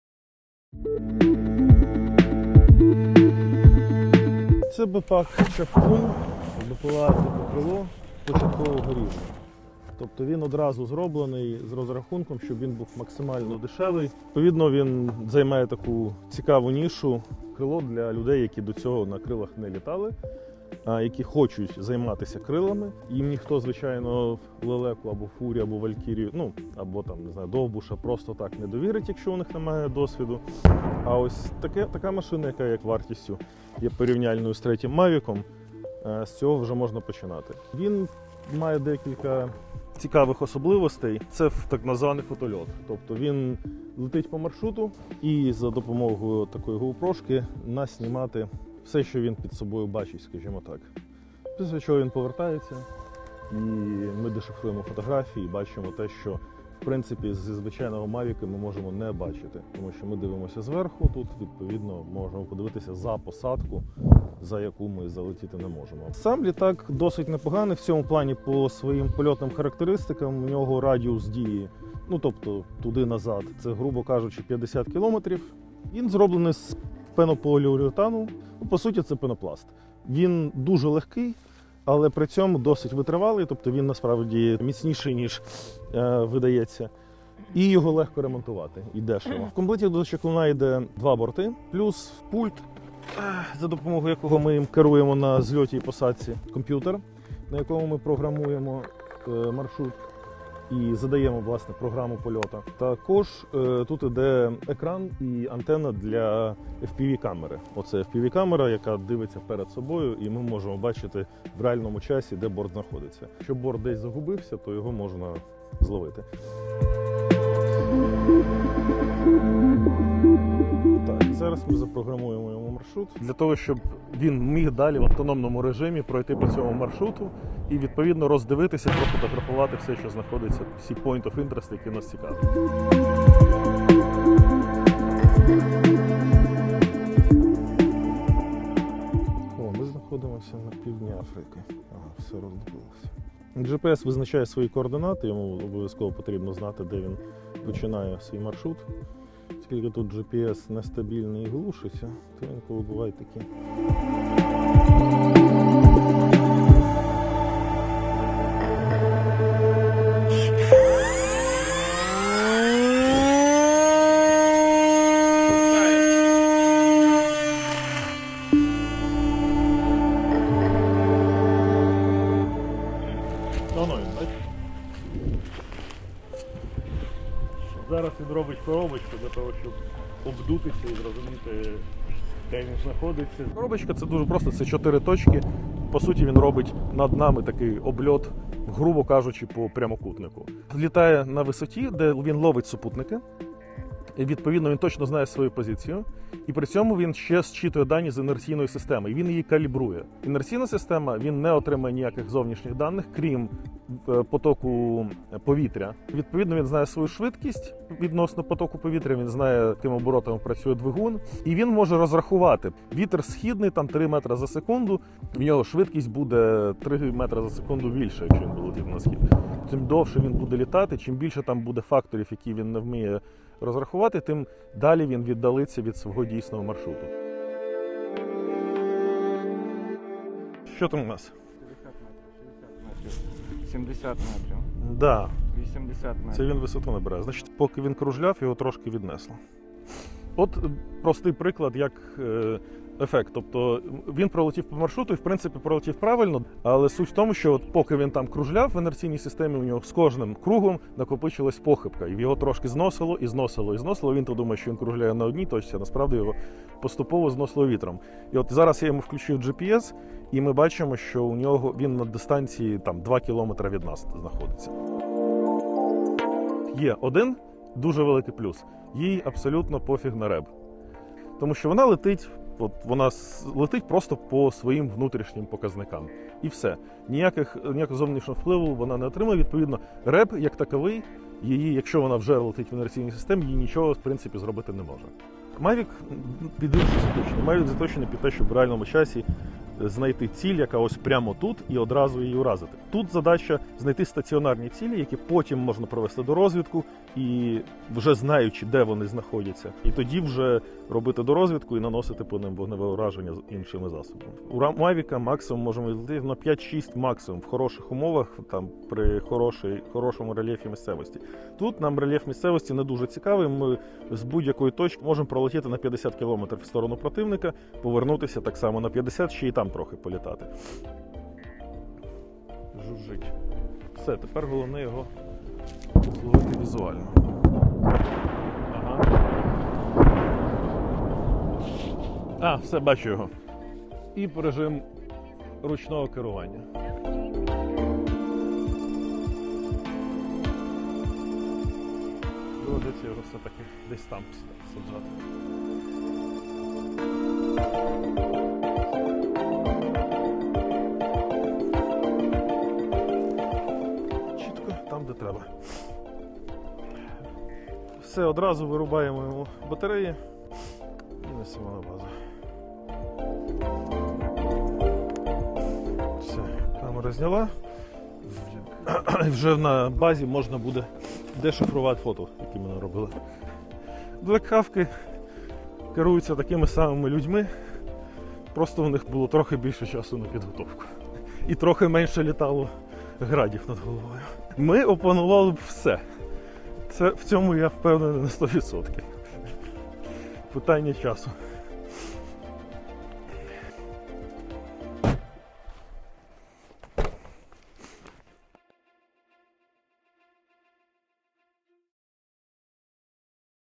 Бійці Луганської бригади Сил ТРО показують, як використовувати безпілотник "Чаклун"
Бійці Луганської бригади Сил територіальної оборони показують, як використовувати безпілотник "Чаклун". "Чаклун" - малий безпілотний розвідувальний авіаційний комплекс літакового типу українського виробництва. Його легко використовувати початківцям.

Джерело: Сили територіальної оборони ЗС України